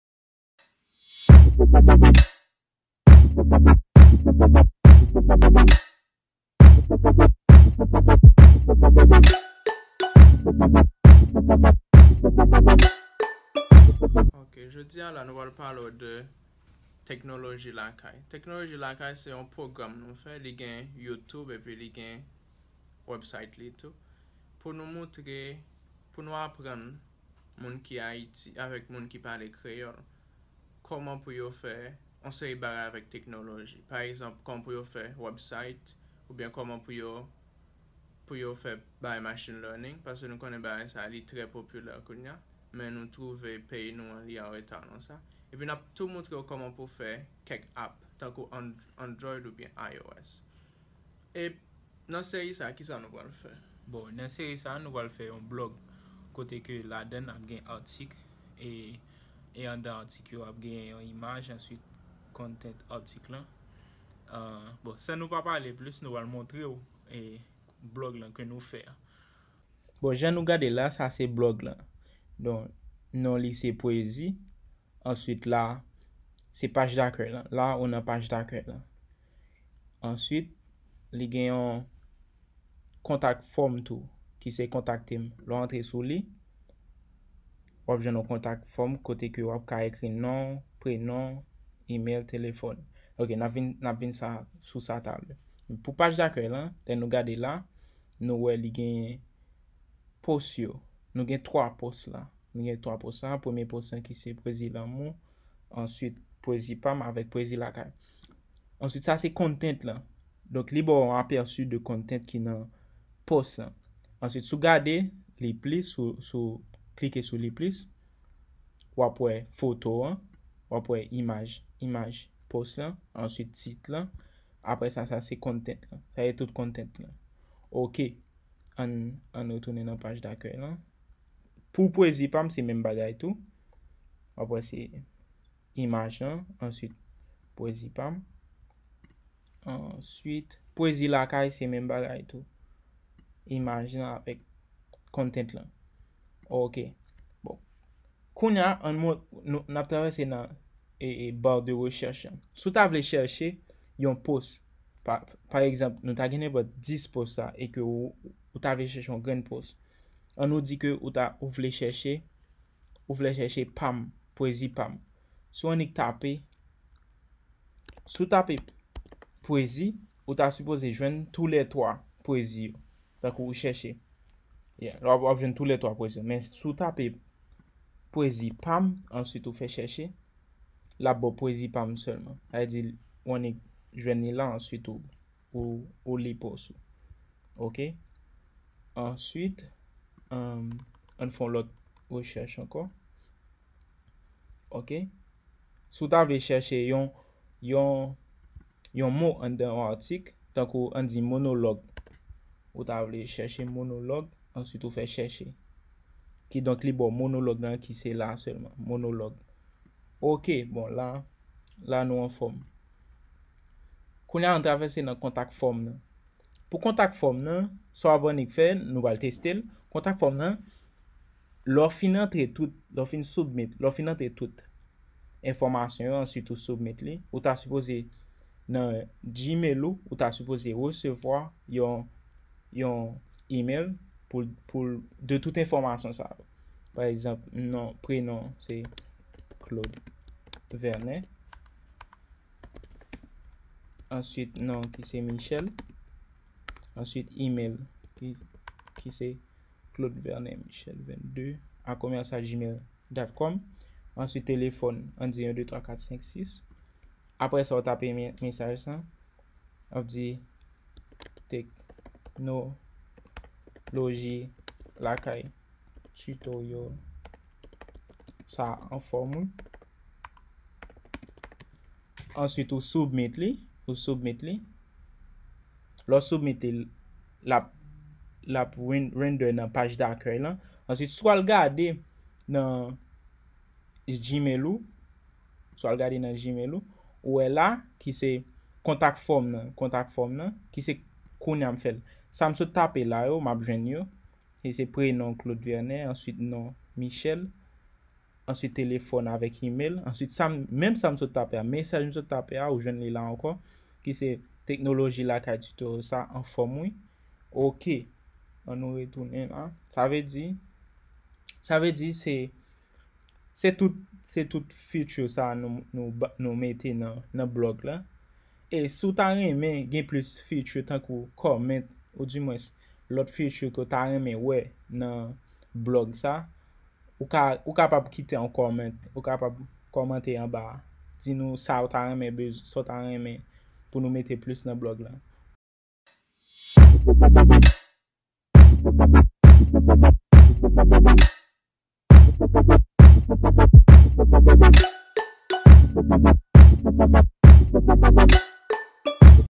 Devlope yon website (Blog) ak Django 3.0 epi Host li nan Heroku - 1/32 Entwodiksyon
Nan seri titoryèl sa, nou pral montre ou koman pou devlope yon Website(Blog) ak Django 3.0 de A jiska Z epi Host li sou Heroku. Pou nou devlope Blog lan nap sèvi ak Python, HTML5, CSS (Bootstrap), JavaScript.